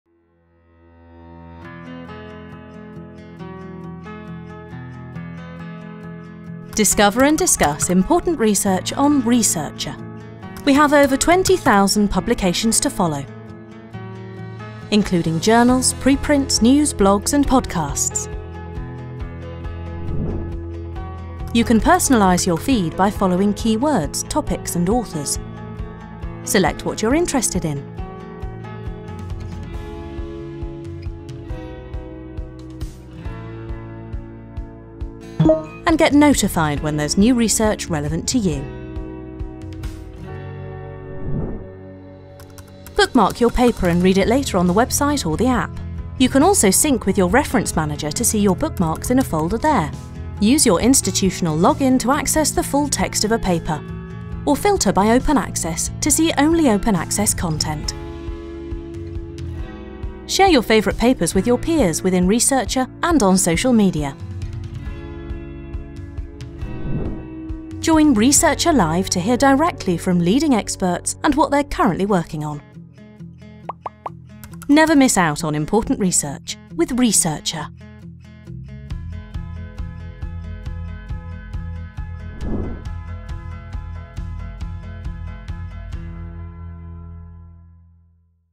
How can Researcher help you with your research?
Researcher is a content discovery and discussion app with over 20,000 content sources and 2.5 million users in the scientific and research communities covering 10 different research areas.

Researcher helps you keep up to date with the latest academic literature, making it easy to stay informed and focus on advancing your research.

Video by @SciAni